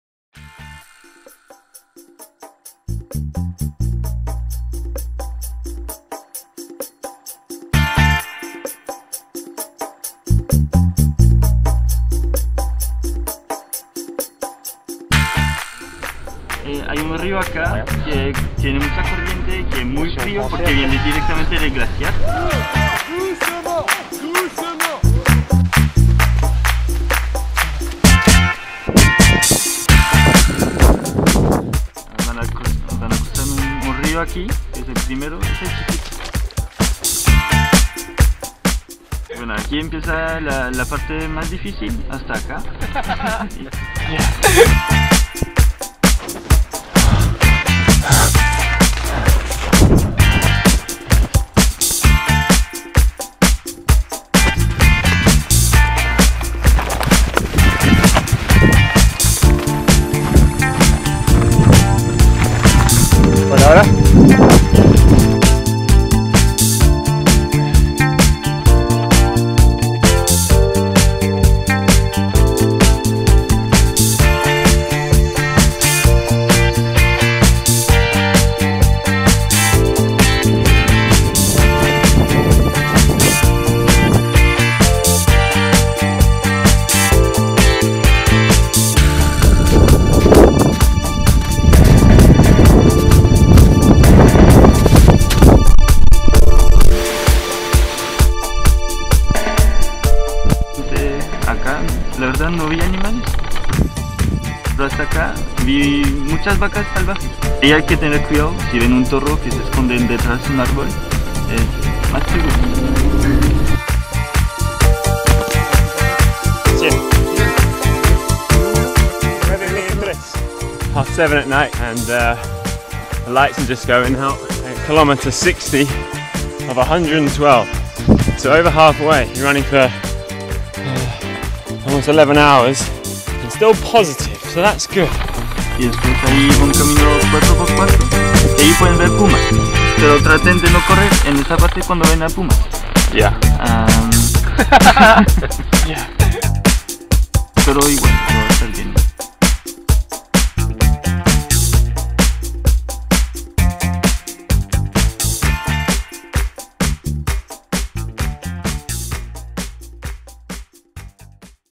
Outdoor Fitness Ultra Fiord 70 Miles
Matt Maynard completed the Patagonian ultra-Fiord.

Remaining unscathed became a key element of what could be called a survival race. Read all about it in the Jan issue of Outdoor Fitness. Out on Dec 18th